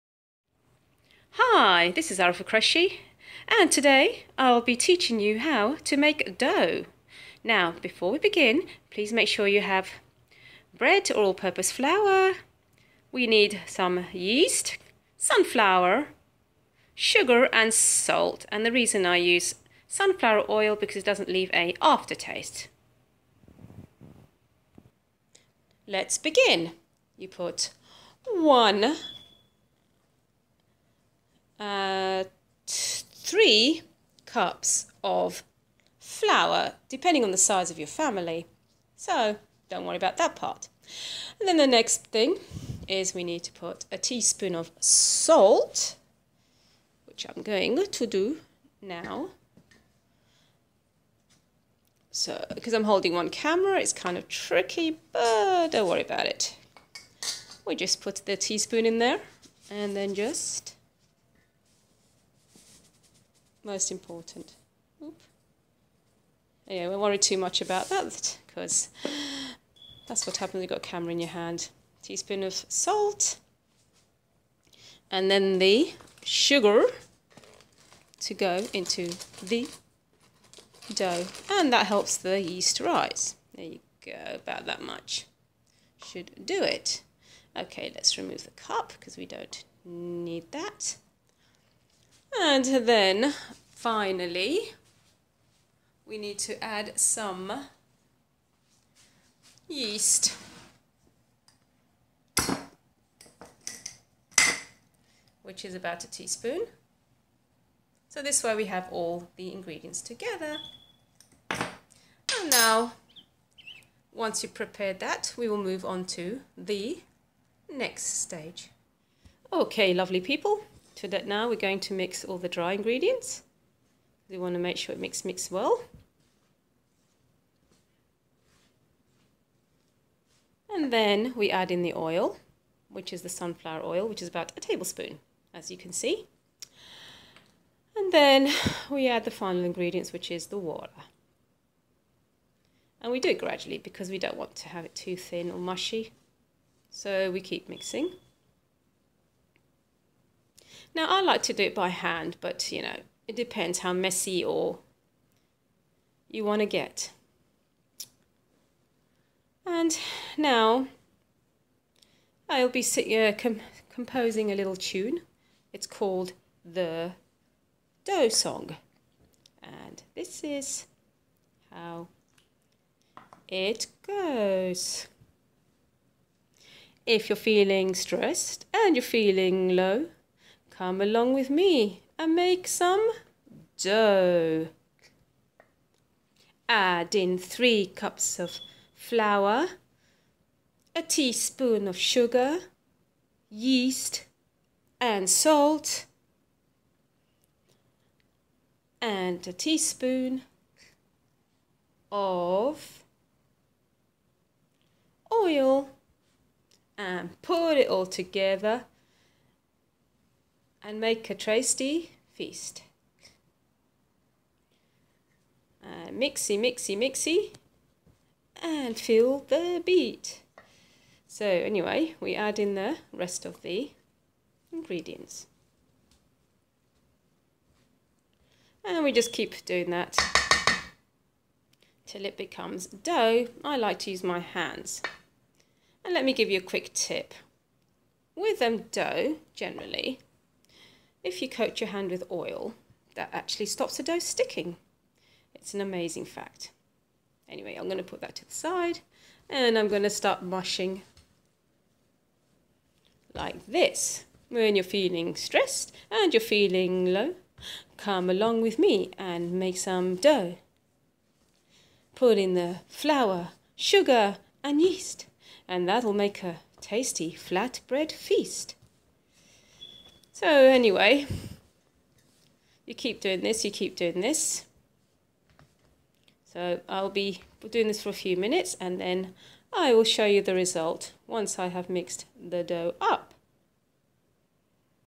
How to make a simple Indian Naan from scratch. Part1
This video is for making Nans for Dummies.